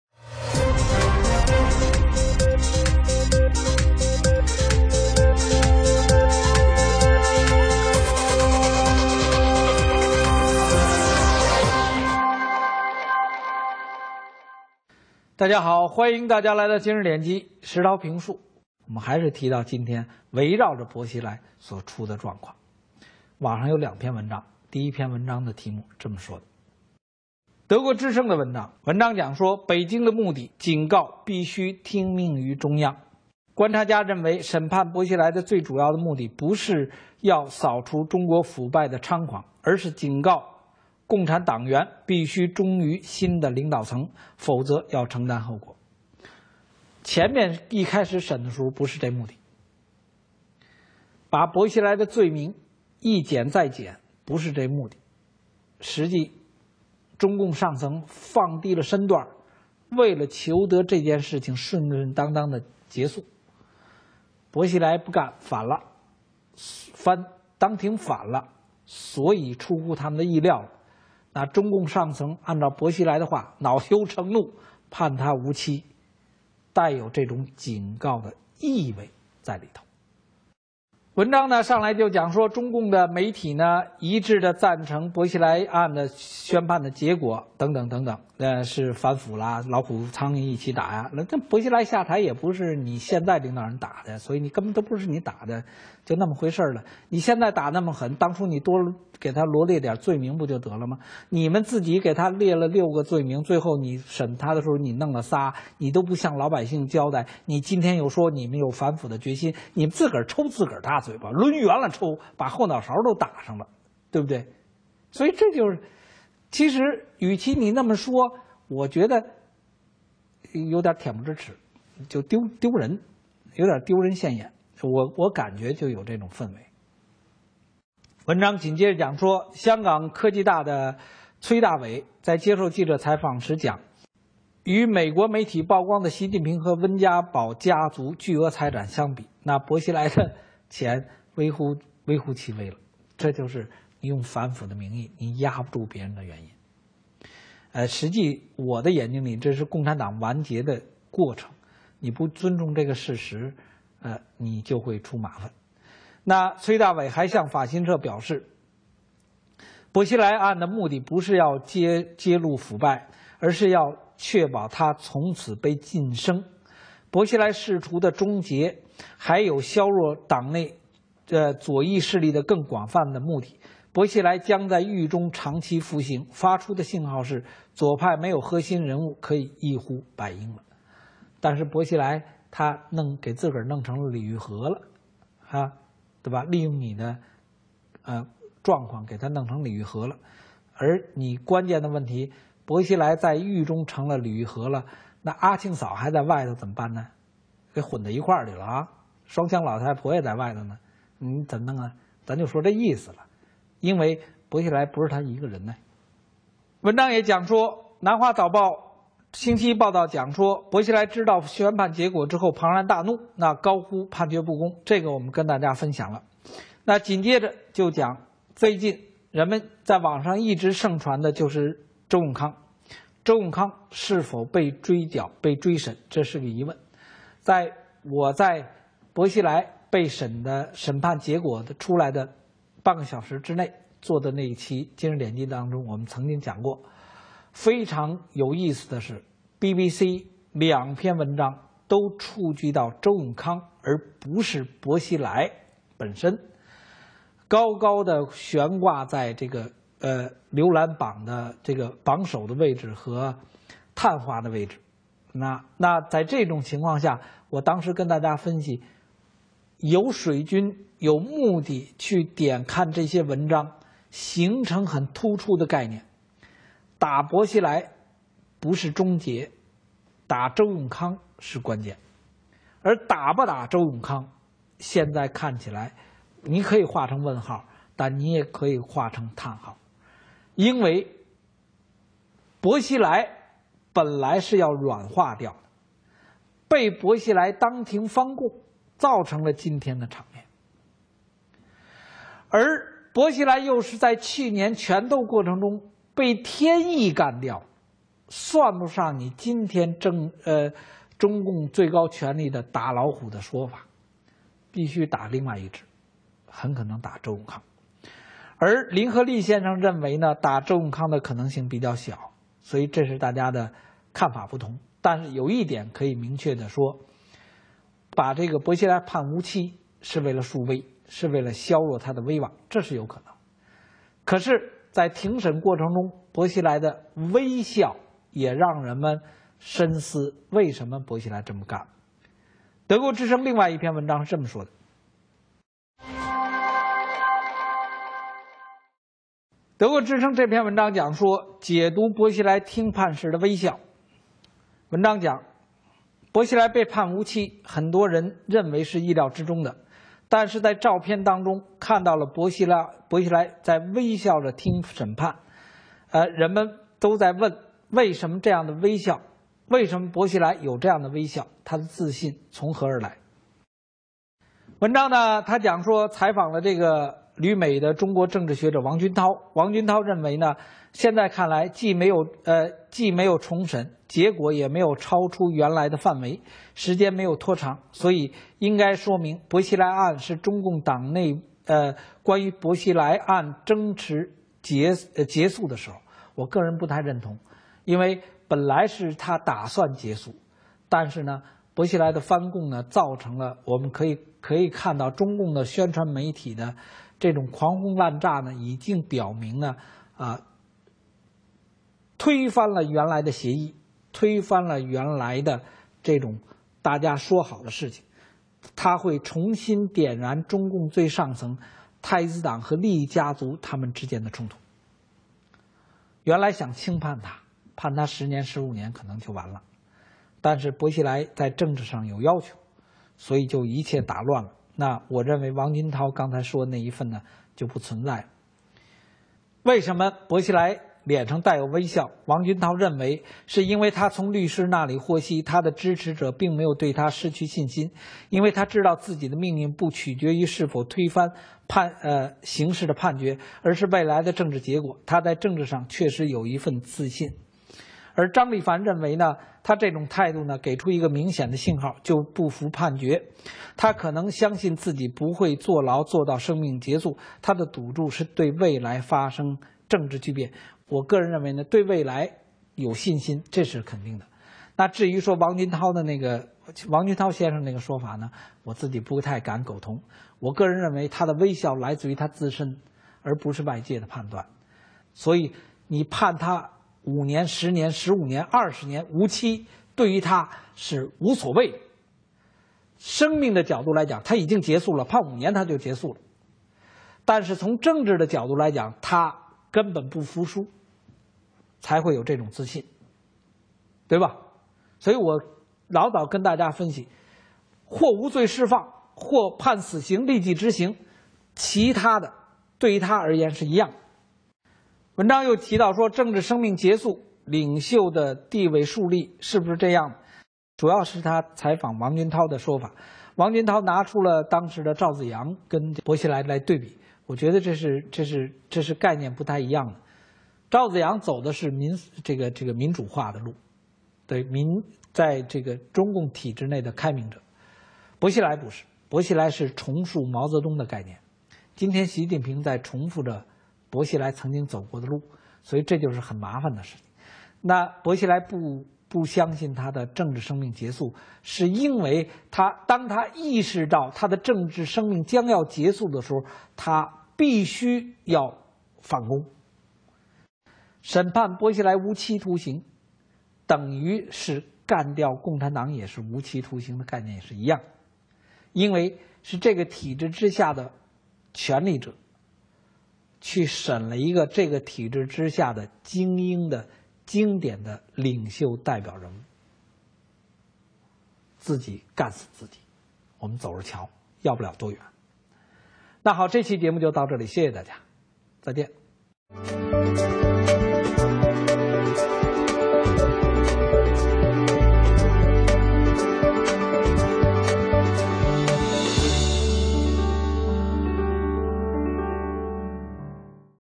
【习近平_石涛评述】薄熙来微笑接无期 习近平警告其他中共高官？
【习近平_石涛评述】薄熙来微笑接无期 习近平警告其他中共高官？(NTDNewsClick) 警告必须听命于中央：薄案的目的不是要揭发腐败，而是要确保他从此噤声。来源：

本則視頻出處：新唐人電視台 (NTDTV)
突破网路封锁看新唐人節目(大陆网友适用)：